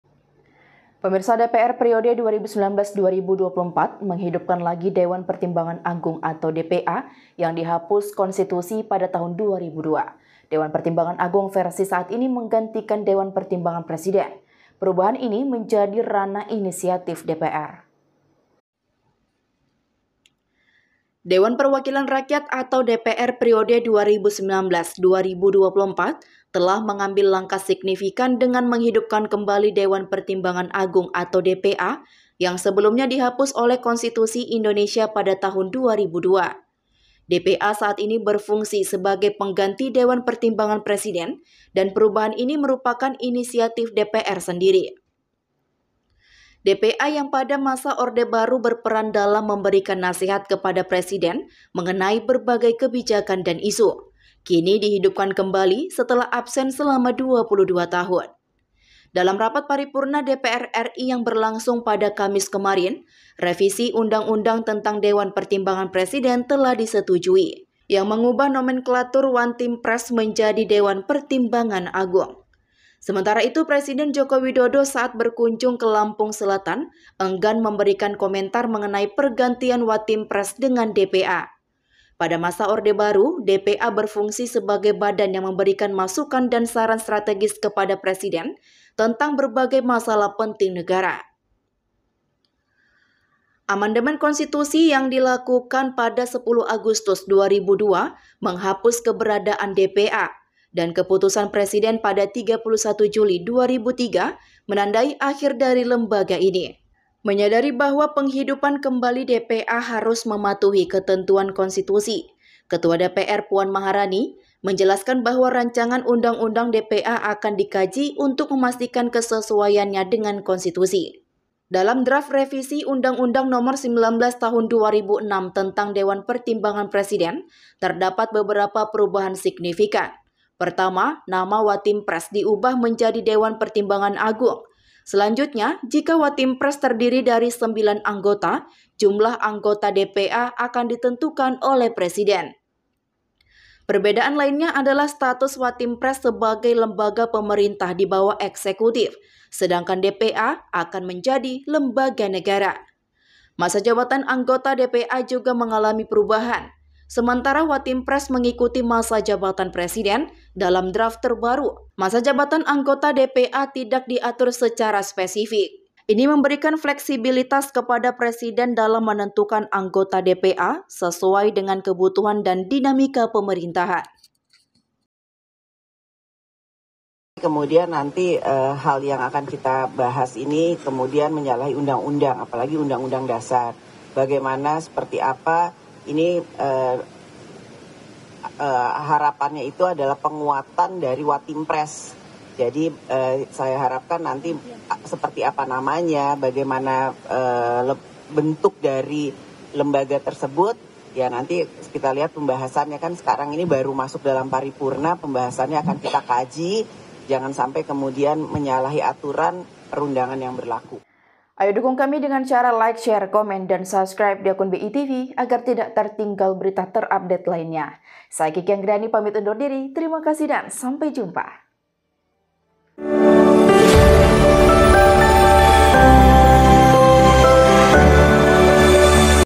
Puan Segera Kaji Rancangan UU DPA Pengganti Wantimpres Supaya Tak Salahi Konstitusi
DPR periode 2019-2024 menghidupkan lagi Dewan Pertimbangan Agung atau DPA yang dihapus konstitusi pada 2002.Dewan Pertimbangan Agung versi saat ini menggantikan Dewan Pertimbangan Presiden. Perubahan ini menjadi ranah inisiatif DPR.